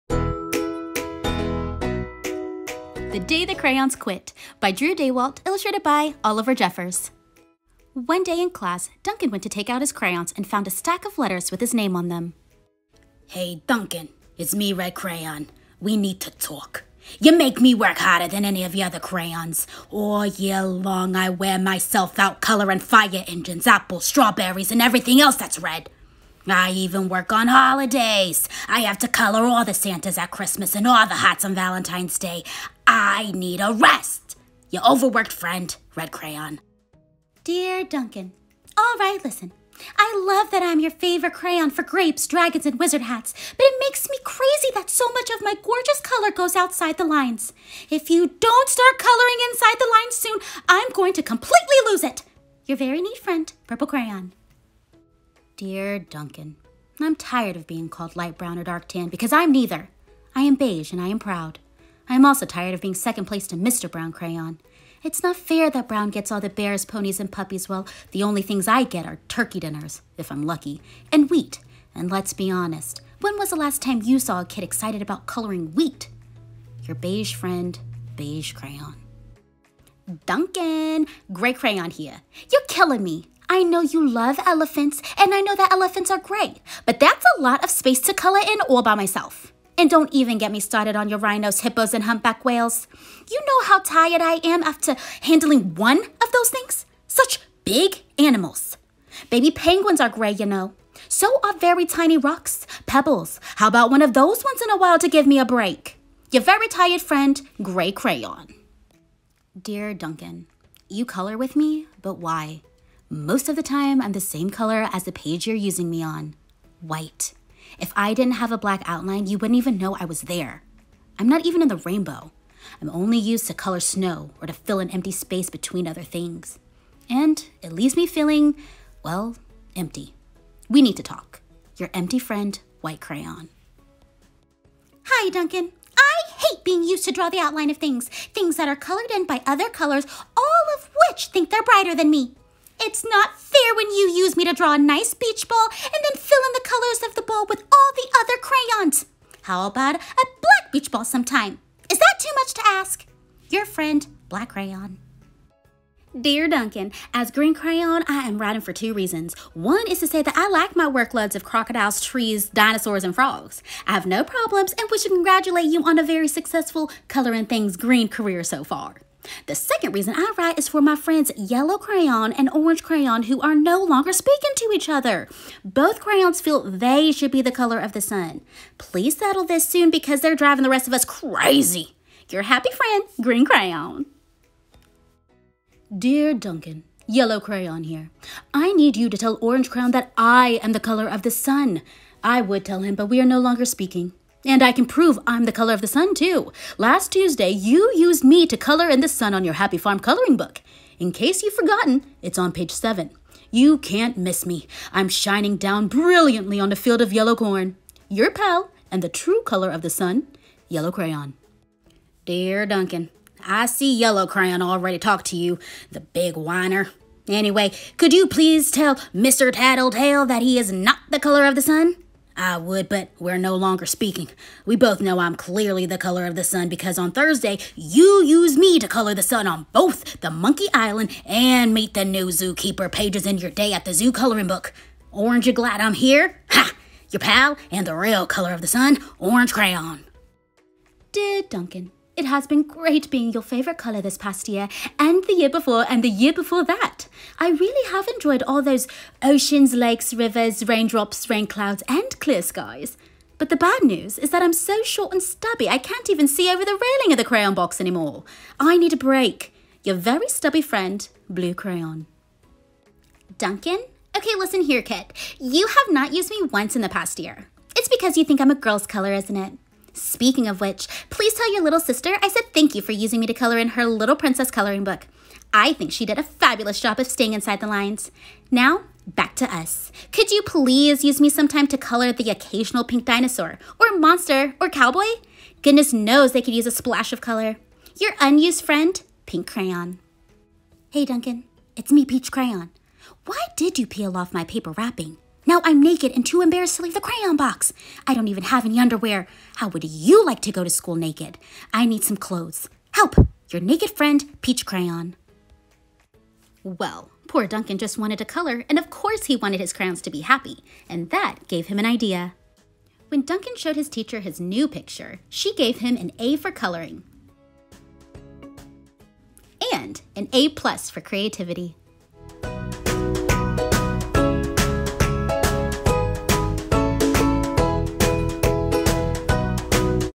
The Day the Crayons Quit read aloud
Duncan’s crayons have had enough with his art! They decide to quit until he agrees to change his ways. What does Duncan do to make things right?